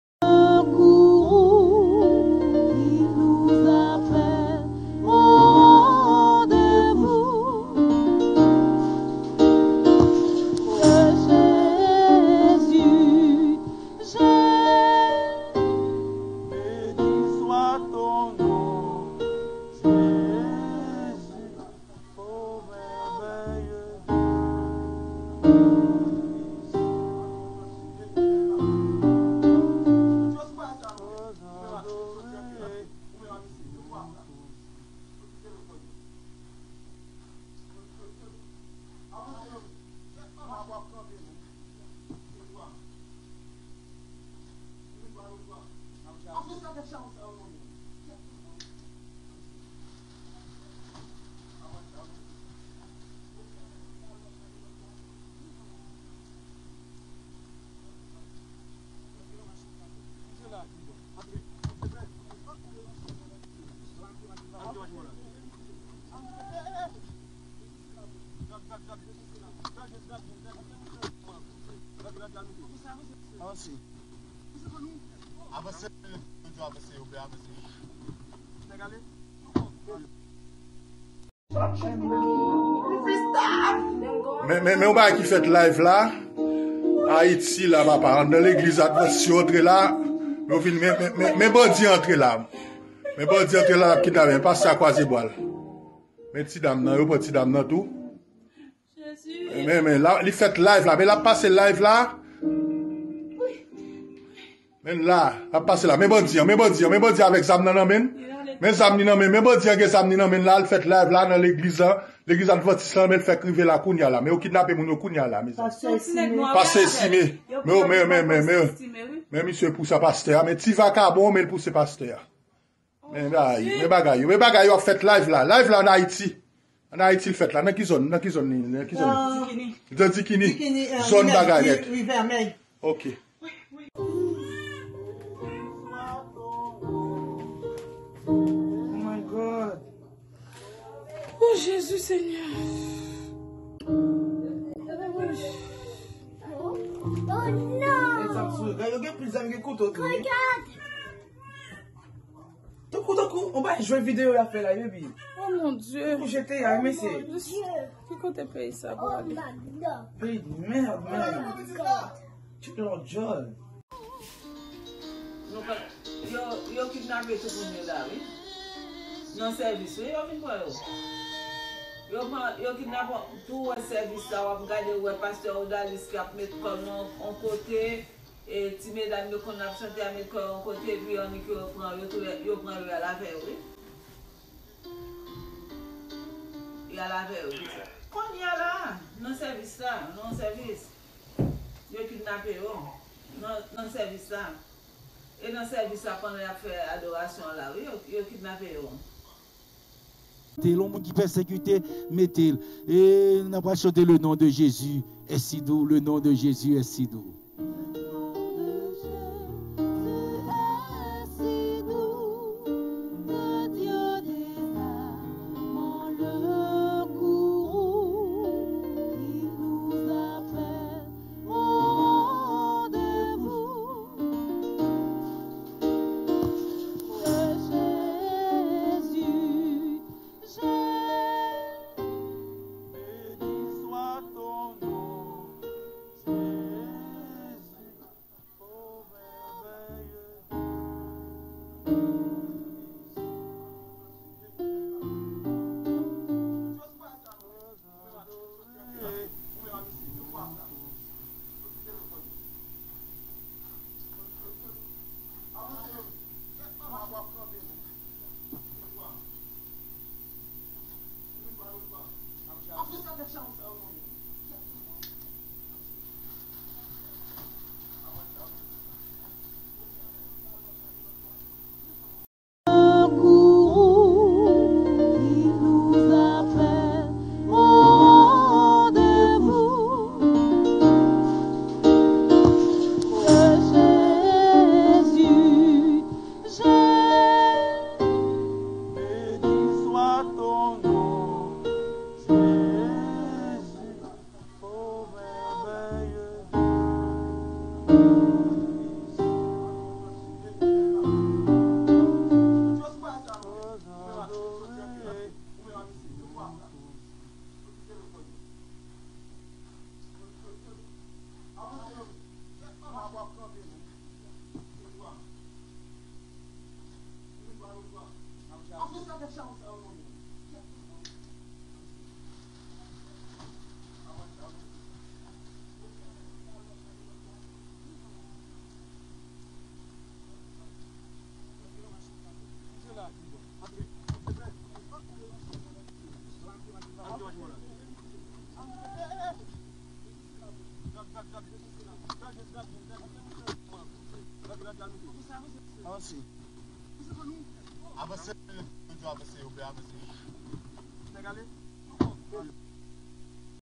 L'ÉGLISE ADVENTISTE du 7ème jour de DIQUINI à P-au-P frappée et ATTAQUÉE par le FLEAU du KIDNAPPING
L'ÉGLISE ADVENTISTE du 7ème jour de DIQUINI à P-au-P frappée et ATTAQUÉE par le FLEAU du KIDNAPPING qui bouleverse le pays tout entier ce Jourd'hui